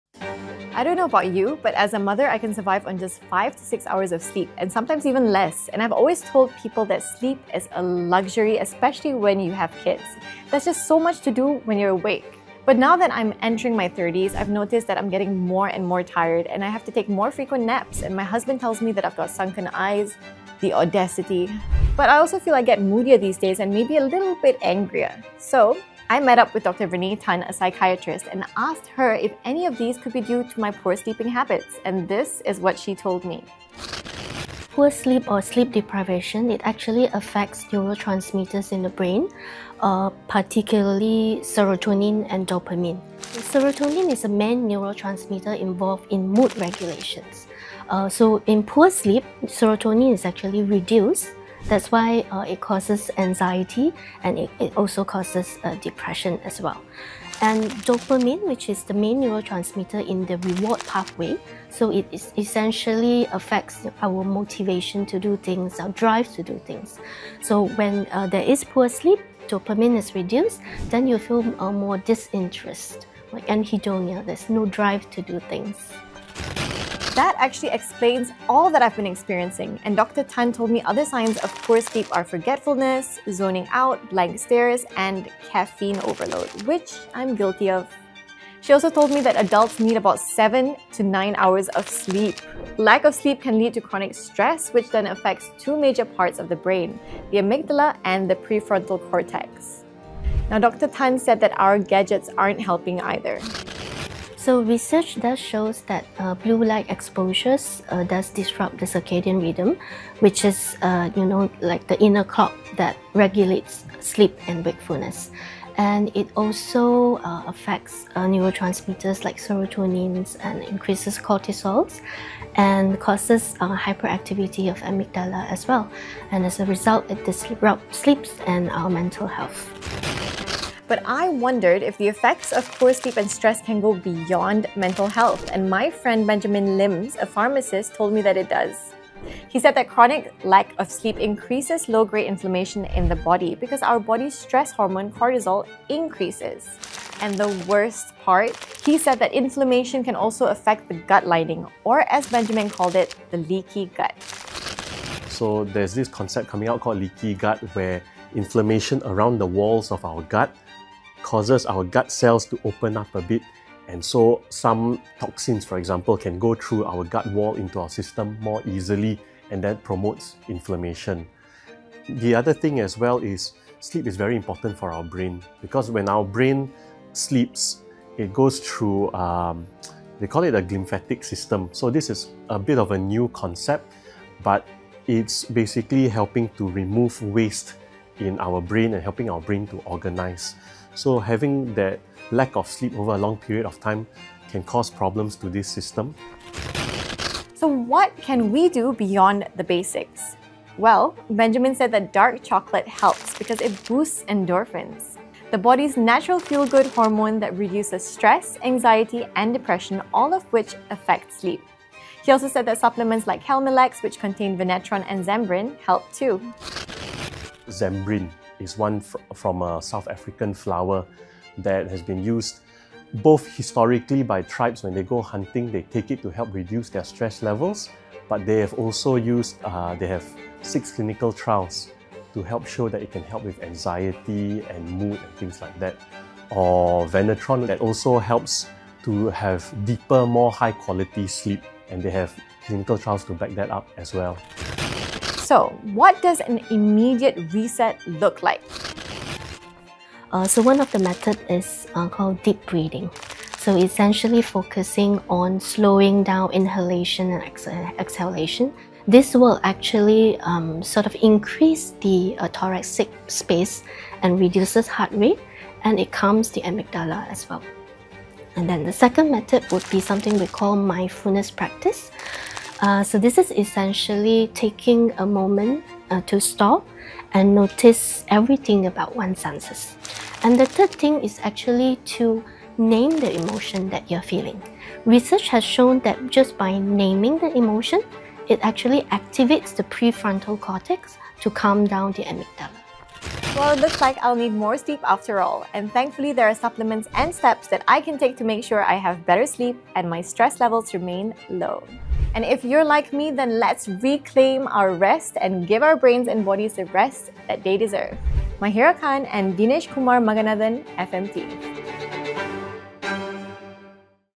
Sleep-deprived and stressed out? Here's what it’s really doing to your brain
Psychiatrist Dr Renee Tan and pharmacist Benjamin Lim reveal how chronic sleep deprivation disrupts brain chemistry, fuels stress, and harms overall health - and share science-backed ways to reset and recover.

Story by: Dinesh Kumar Maganathan
Shot by: Hizami Safri
Presented by: Mahirah Khan
Edited by: Kiera Amin